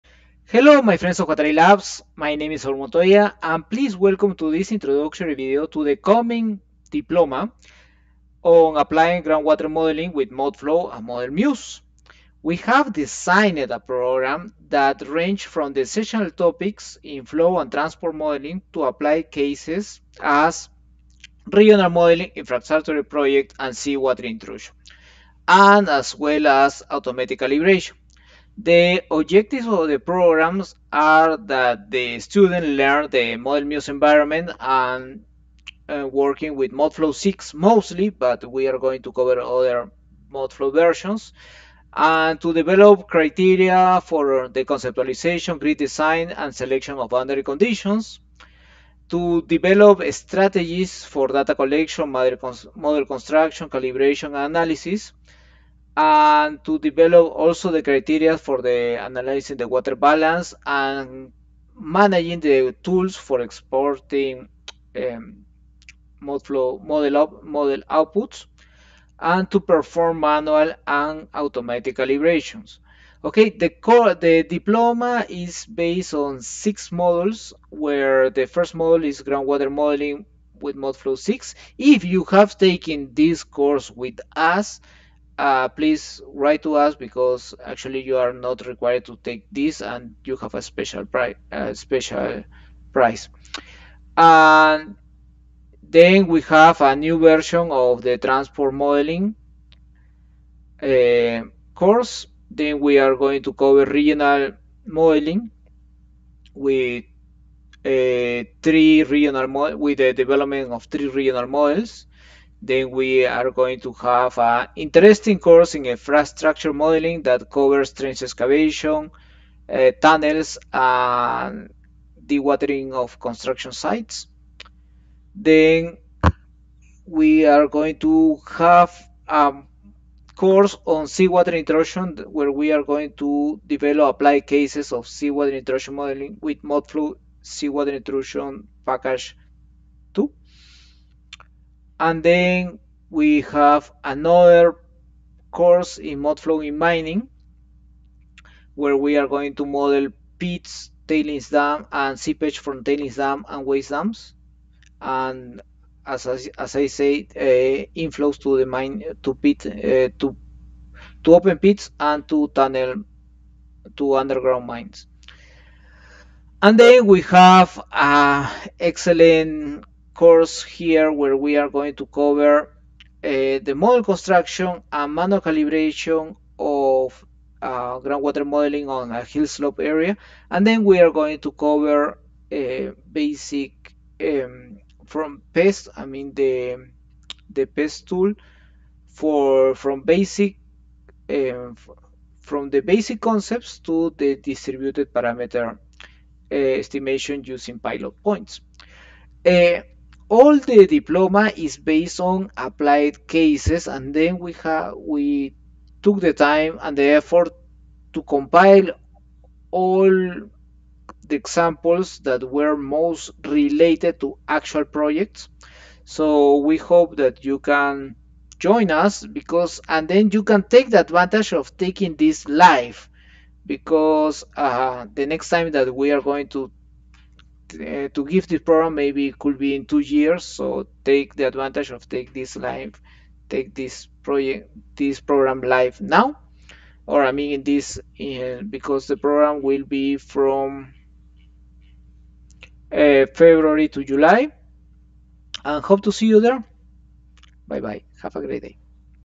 Intro to the Diploma in Applied Groundwater Modeling with MODFLOW and Model Muse - Feb to Jul 2023
We have designed a complete program on MODFLOW with Model Muse that ranges from the essential topics in flow and transport modeling to applied cases of regional modeling, infrastructure projects, and seawater intrusion. MODFLOW with Model Muse is a recent open-source software that provides various tools for simulating the groundwater flow regime in different environments.